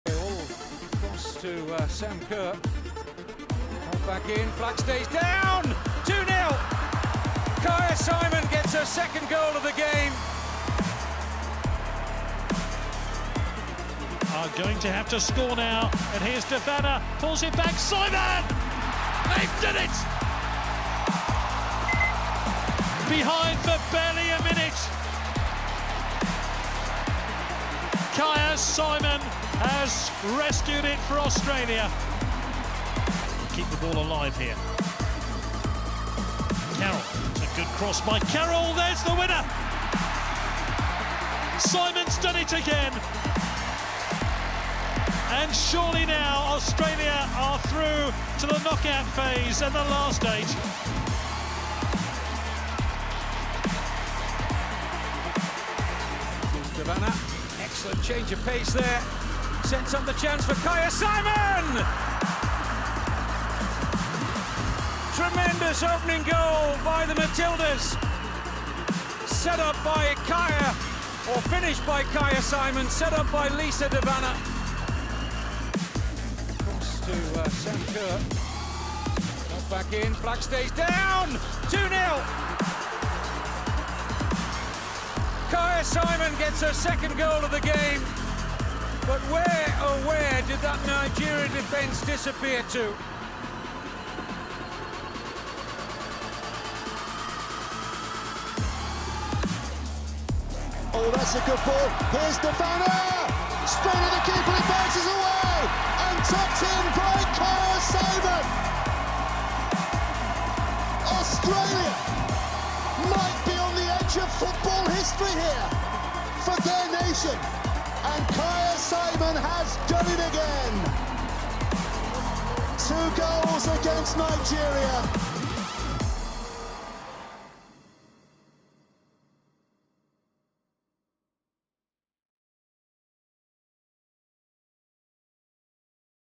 🇦🇺 Kyah Simon | FIFA Women's World Cup Goals
She's scored some big goals for the Matildas on the global stage. Take a look back at all of Kyah Simon's goals for Australia at the FIFA Women's World Cup.